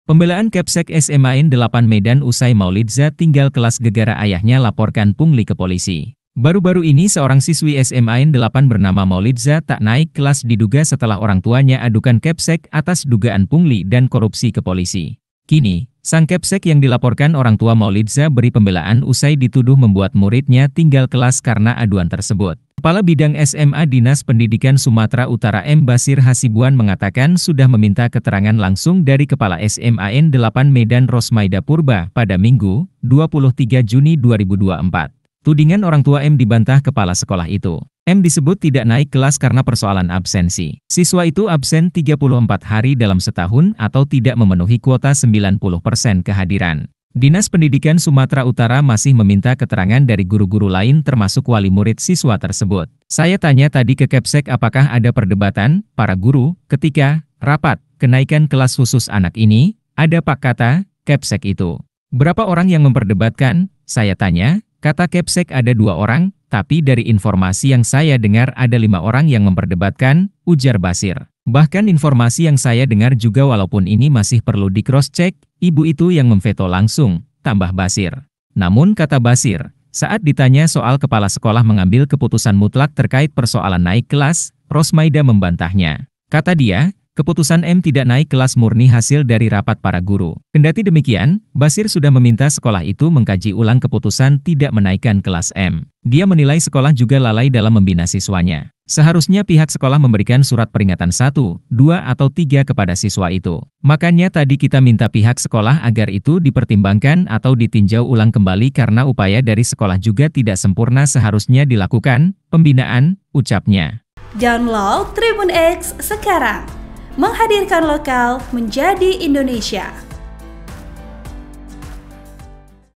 PEMBELAAN Kepsek SMAN 8 Medan Usai Maulidza Tinggal Kelas Gegara Ayahnya Laporkan Pungli ke Polisi
Berikut pembelaan kepala sekolah SMAN 8 Medan setelah muridnya tinggal kelas diduga karena orangtuanya laporkan pungli dan korupsi ke polisi.
Baru-baru ini seorang siswi SMAN 8 bernama Maulidza tak naik kelas diduga setelah orangtua nya adukan kepsek atas dugaan pungli dan korupsi ke polisi.

Program: REGIONAL
Editor Video: dicky